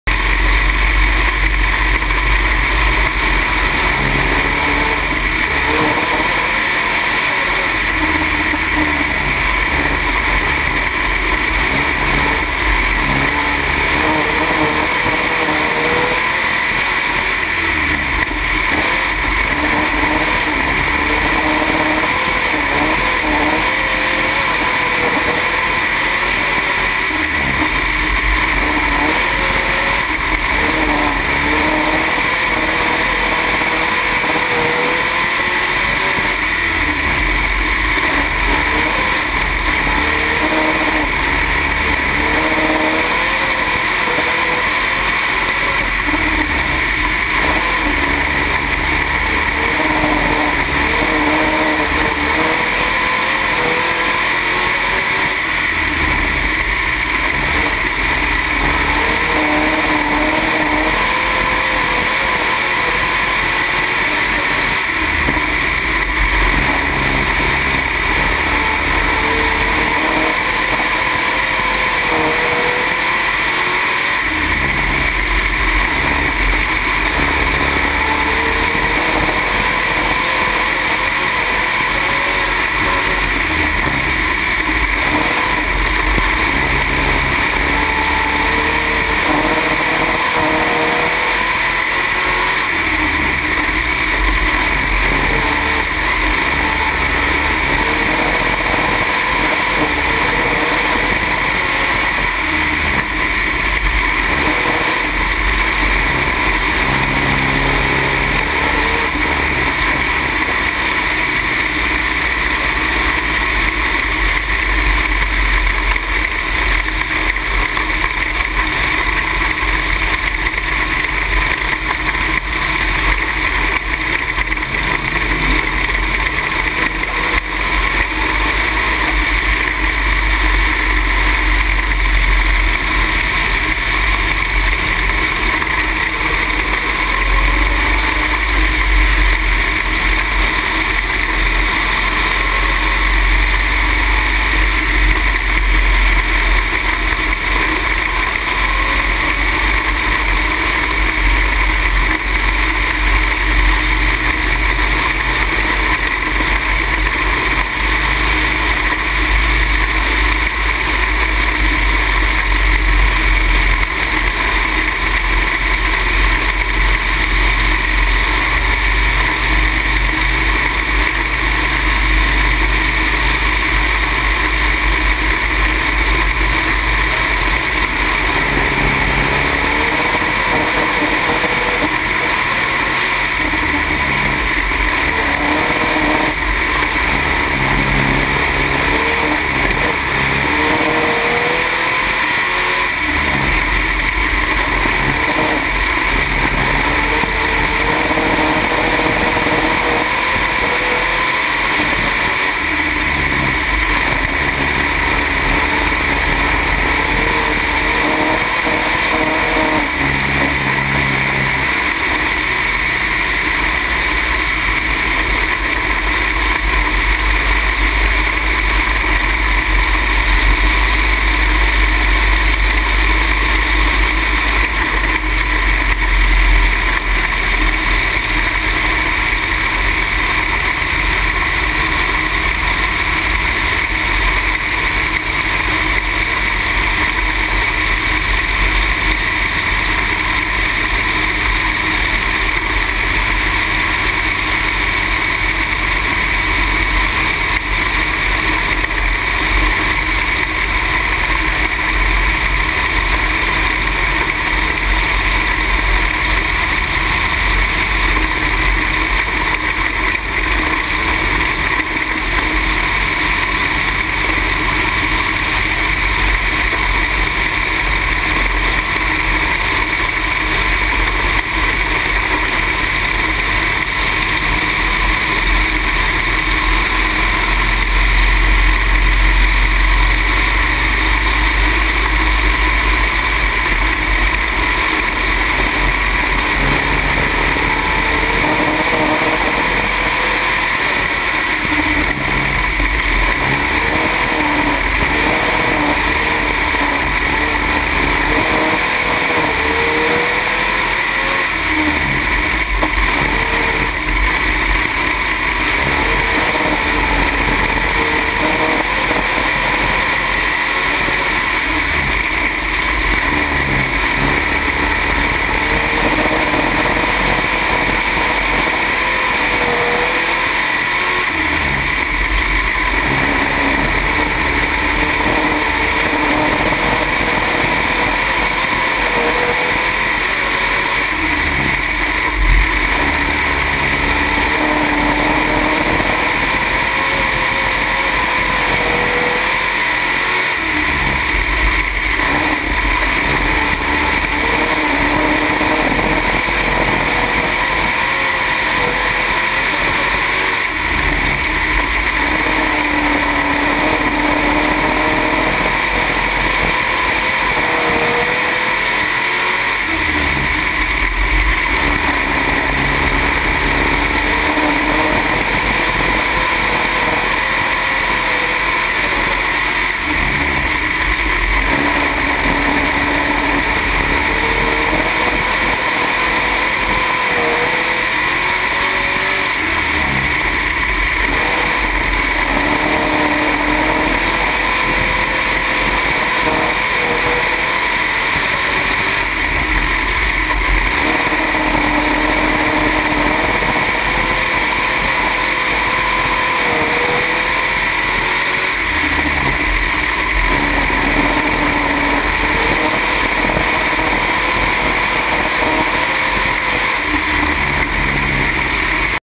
henkins racing Elkins raceway feature race go pro view
Dylan in the Blue Line Racing Stables 3D Feature race his go pro view at Elkins Raceway